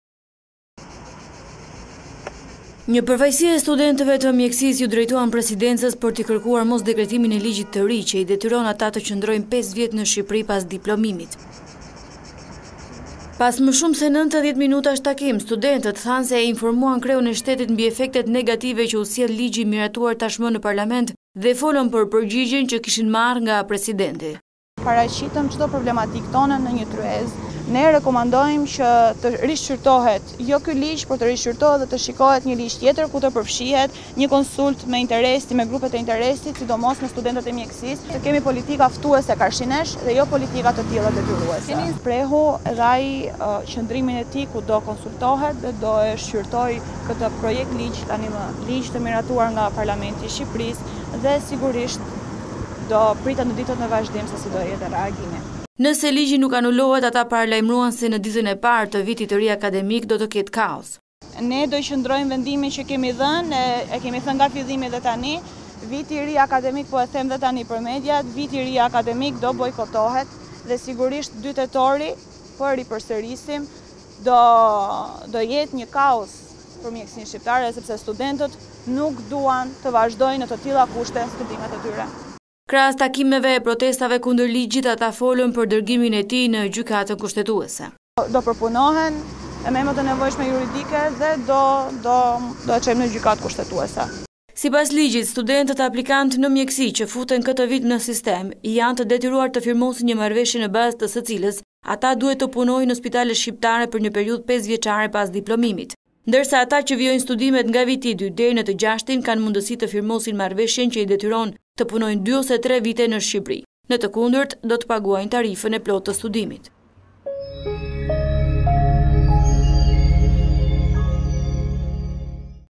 Report TV - Diploma peng për 5 vite, studentët e Mjekësisë i shkojnë në zyrë Presidentit Begaj
reportTV lajmeshqip lajme Studentët e Mjekësisë i shkuan sot në zyrë presidentit Bajram Begaj për ti kërkuar të mos dekretojë ligjin që i detyron ata të kryejnë deri në 5 vite punë para se të marrin diplomën.